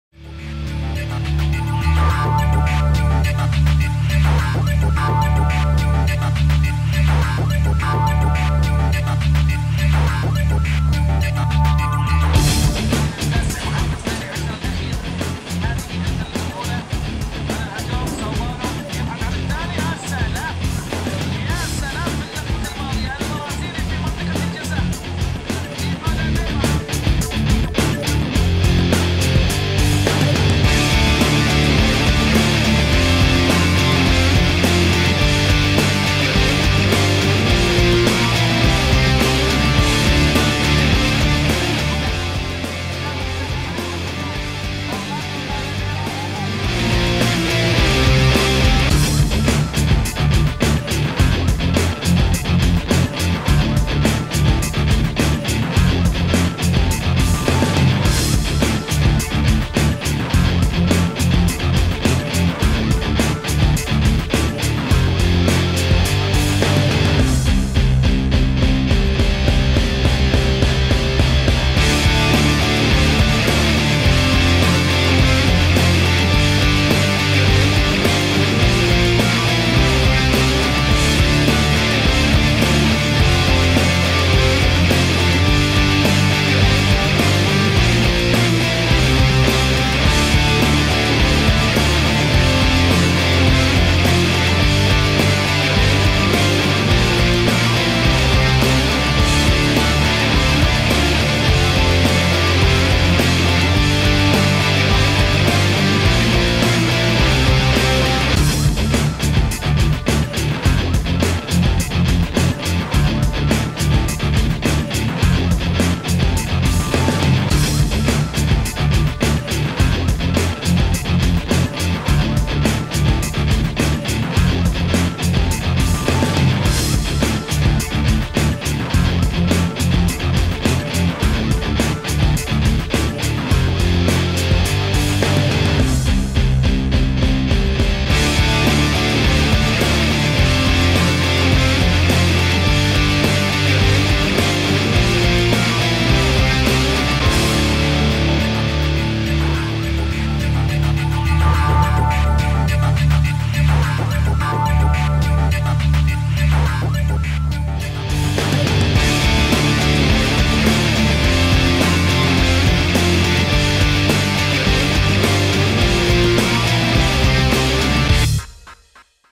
Augusto Galvan - Sao Paolo Fc
Ένα από τα ανερχόμενα ταλέντα του Βραζιλιάνικου ποδοσφαίρου έχει βάλει στο "μάτι" η Ρεάλ Μαδρίτης. Ο λόγος για τον 17χρονο άσο της Σάο Πάολο, Αουγκούστο Γκαλβάν. Πάρτε μια γεύση από το ταλέντο του.